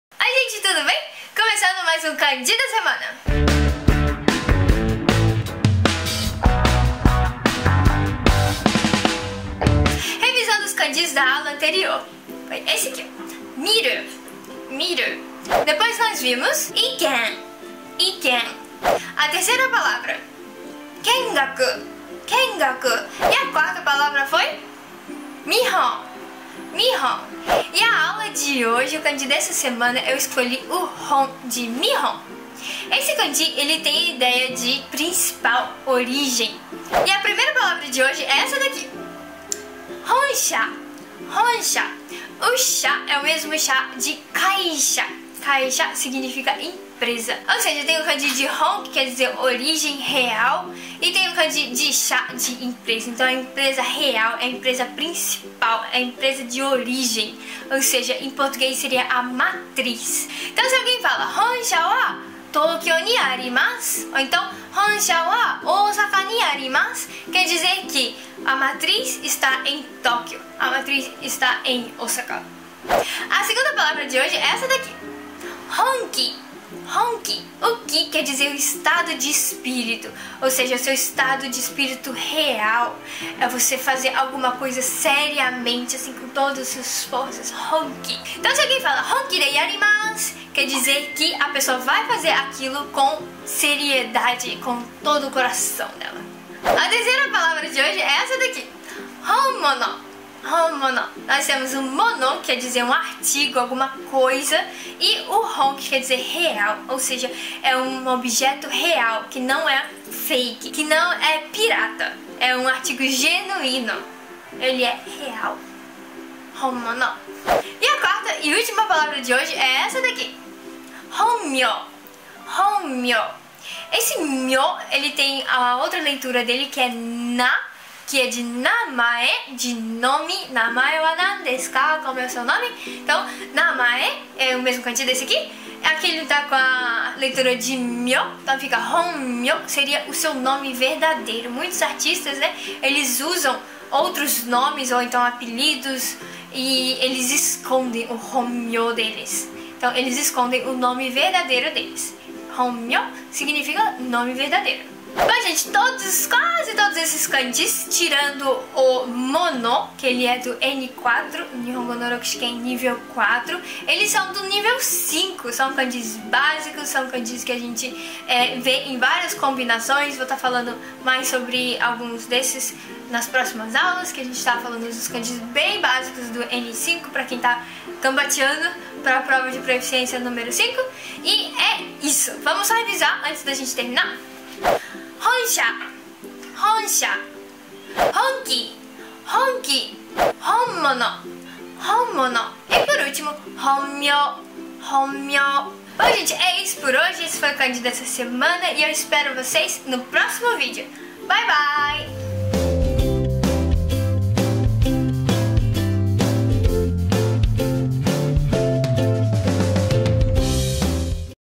HON - KANJI DA SEMANA #16
O que aprendemos na aula 16

Nesta aula falei sobre os kanjis que vemos com frequência na porta dos restaurantes aqui no Japão. Já pensou está escrito 準備中 e você já sair entrando para pedir o seu almoço? 😱😱

Pegue um lápis e o seu caderninho nihongando e já anote aí os kanjis

本社 - honsha
本気 - honki
本物 - honmono
本名 - honmyou

Venha fazer parte do canal do telegram e ter acesso a conteúdos gratuitos e exclusivos:

Para quem quer ir além no japonês, acesse aqui para agarrar esses materiais TOPS!

*  EDITADO POR AKAMINEX - SOLUÇÃO EM MÍDIA DIGITAL           *